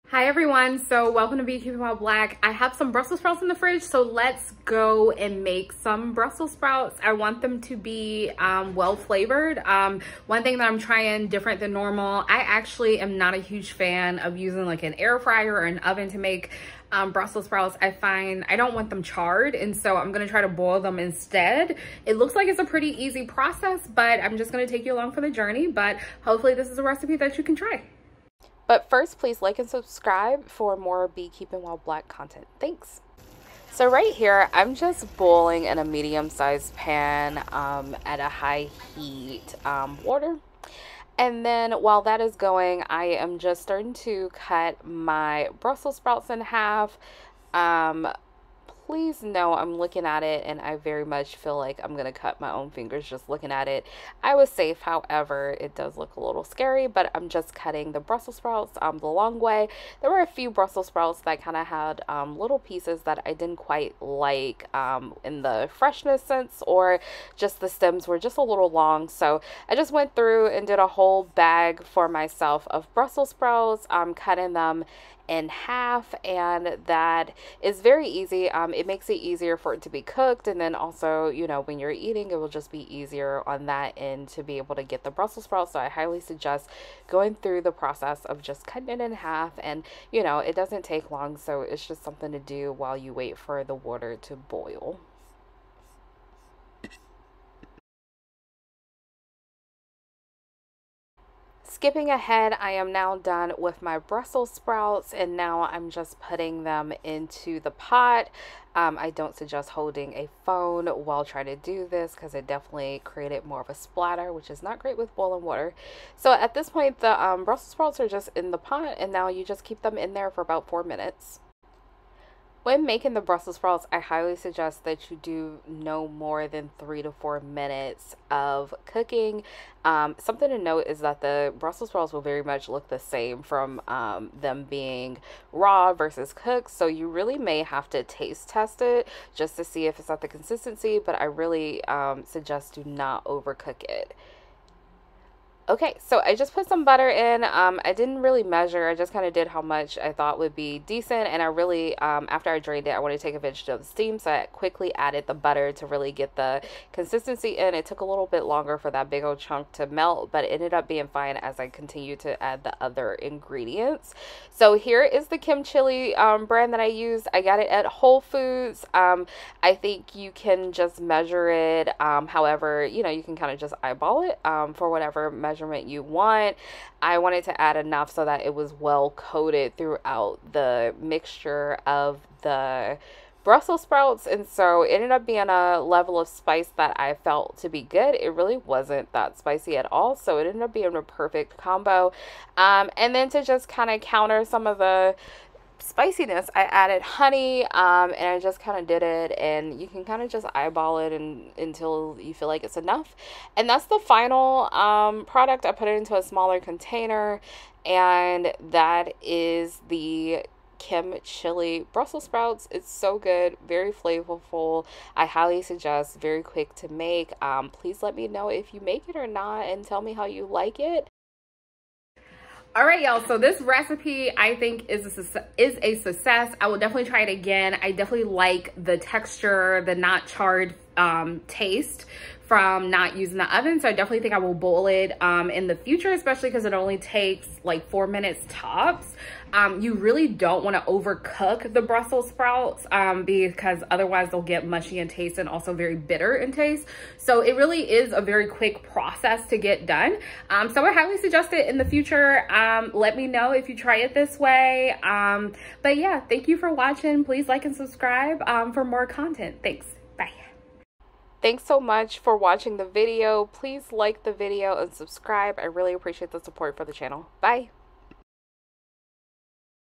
Kimchili Honey Brussel Sprouts Recipe
Kimchili Brussel Sprouts are so good and easy to make.  Let me know if you try the recipe!